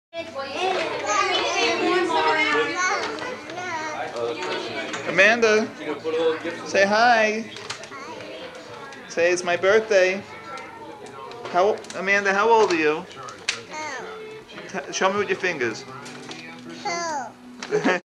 Amanda Is 2
Amanda tells us how old she is before going shopping at Prada.